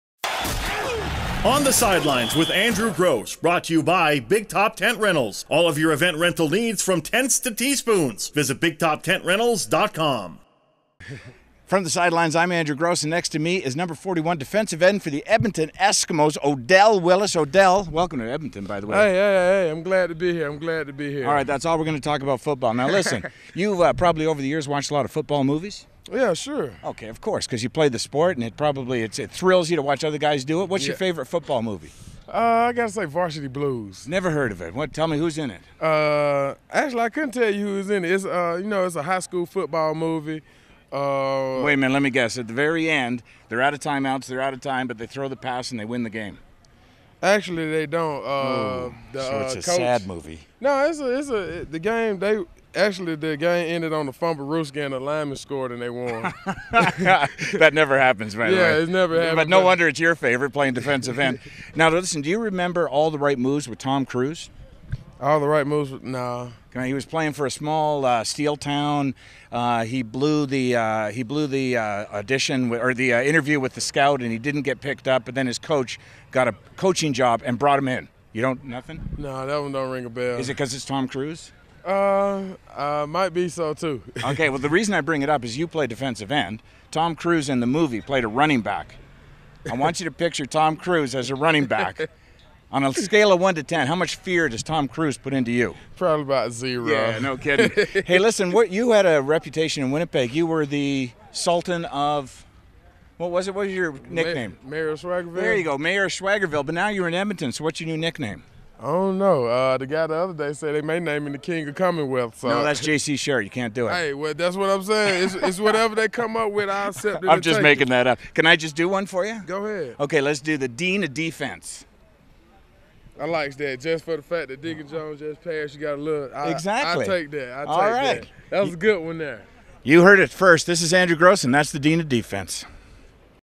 #41 Odell Willis - On the Sidelines 2013
Andrew Grose talks with Edmonton Eskimos DE, Odell Willis